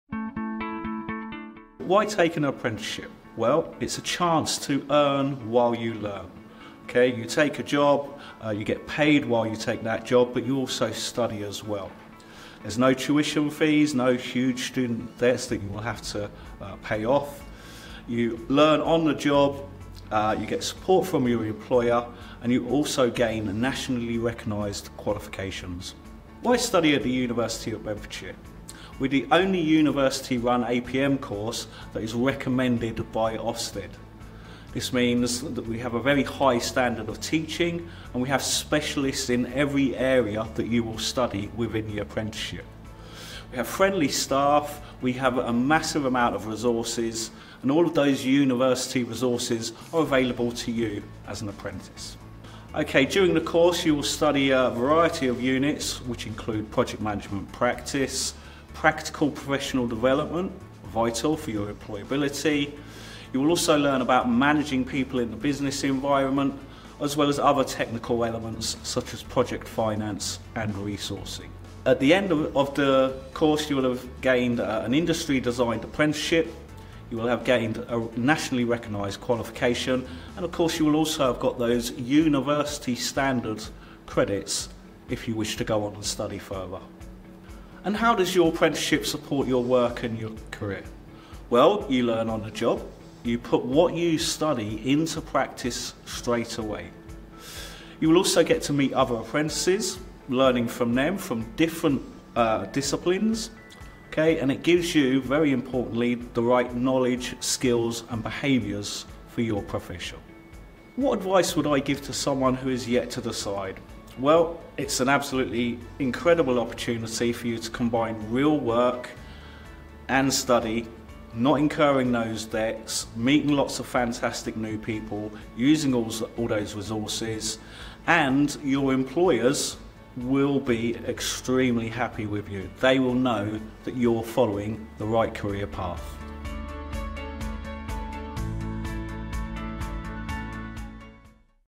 Hear from the Course Coordinator for the Associate Project Manager Apprenticeship
Join Lee Smith-Dennis as he delves into the perks of apprenticeships and why the University of Bedfordshire stands out as the ultimate choice for aspiring project managers.